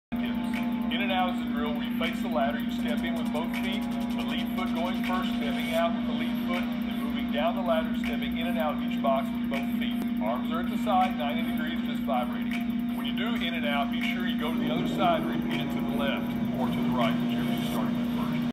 Speed ladder in & out
Speed ladder drill, both feet in, both feet out